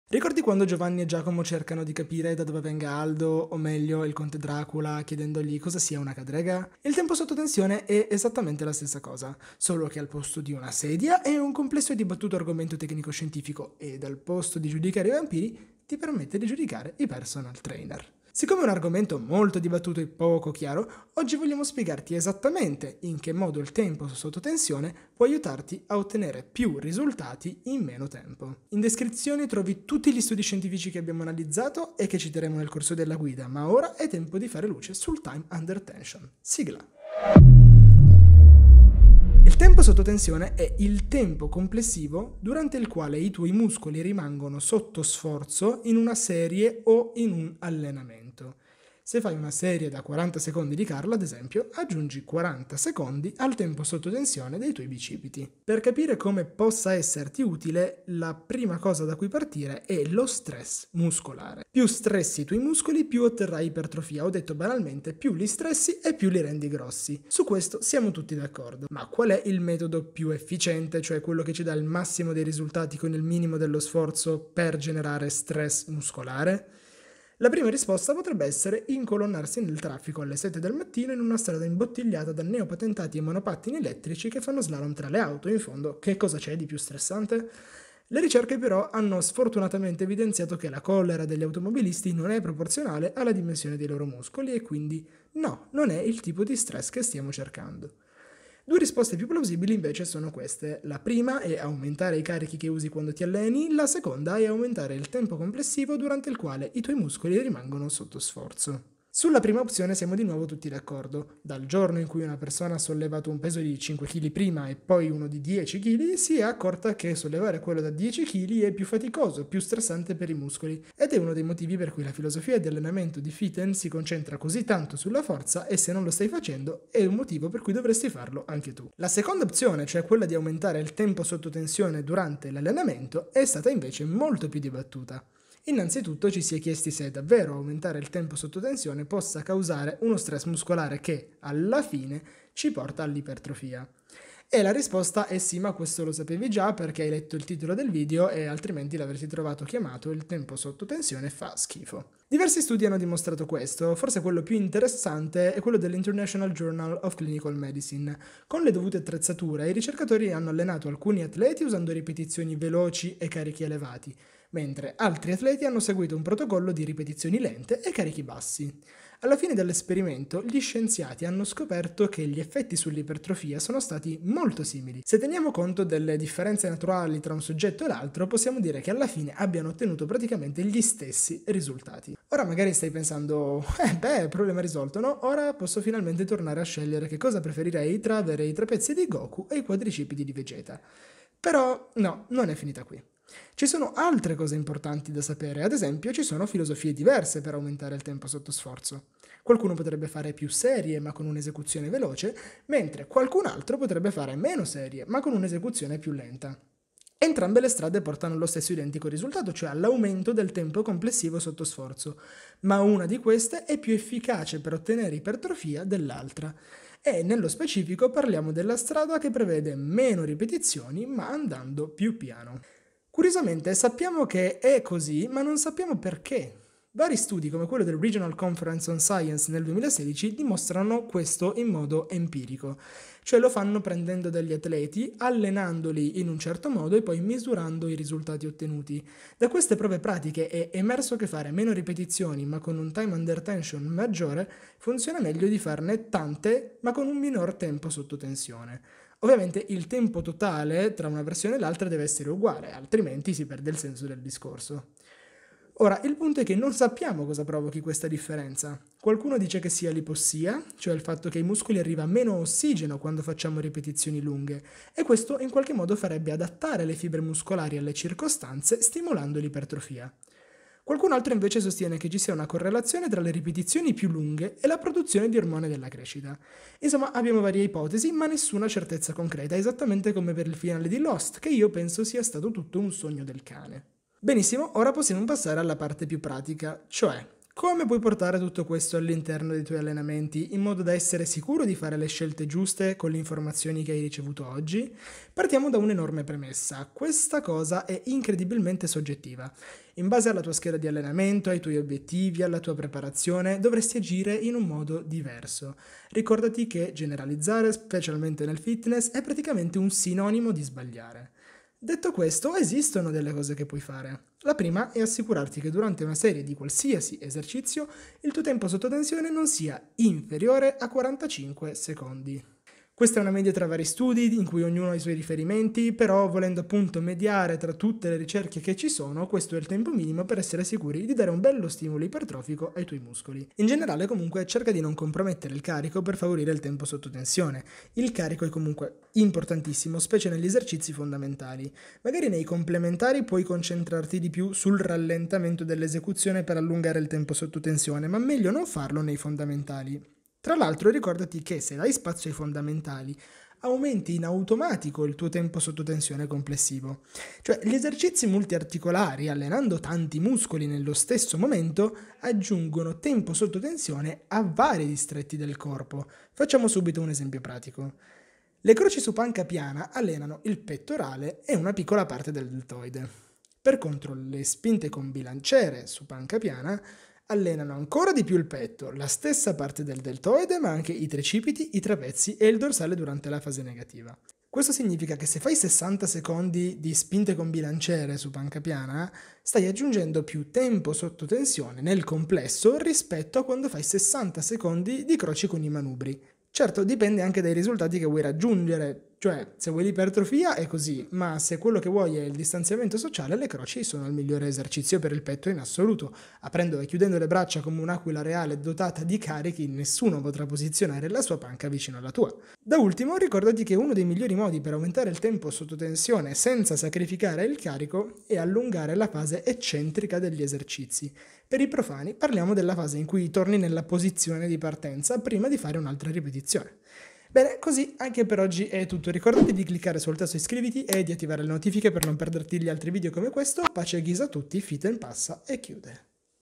Scolpisci il Fisico gestendo il Time Under Tension (TUT) degli Allenamenti
Il Time-Under-Tension, cioè il tempo sotto tensione o più semplicemente "TUT", è un elemento importante di cui tenere conto negli allenamenti. Nel video parliamo di come gestire questo fattore al meglio per generare ipertrofia e per ottenere il massimo dei benefici da ogni esercizio.

Fonti e studi nominati o solo utilizzati:

1) Burd, N. A., Andrews, R. J., West, D. W., Little, J. P., Cochran, A. J., Hector, A. J., ... & Phillips, S. M. (2012). "Muscle time under tension during resistance exercise stimulates differential muscle protein sub fractional synthetic responses in men". The Journal of Physiology, 590(2), 351-362.

2) Mohamad N.I., Nosaka K., Cronin J. (2016) Kinematics and Kinetics of High and Low Velocity Resistance Training Equated by Time Under Tension: Implications for Hypertrophy Training. In: Yacob N., Mohamed M., Megat Hanafiah M. (eds) Regional Conference on Science, Technology and Social Sciences (RCSTSS 2014). Springer, Singapore

3) Ogasawara R, Loenneke JP, Thiebaud RS, et al. Low-load bench press training to fatigue results in muscle hypertrophy similar to high-load bench press training. Int J Clin Med. 2013;4:114–21.

4) Mitchell, Cameron J. et al. “Resistance Exercise Load Does Not Determine Training-Mediated Hypertrophic Gains in Young Men.” Journal of Applied Physiology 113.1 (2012): 71–77. PMC. Web. 28 Nov. 2017.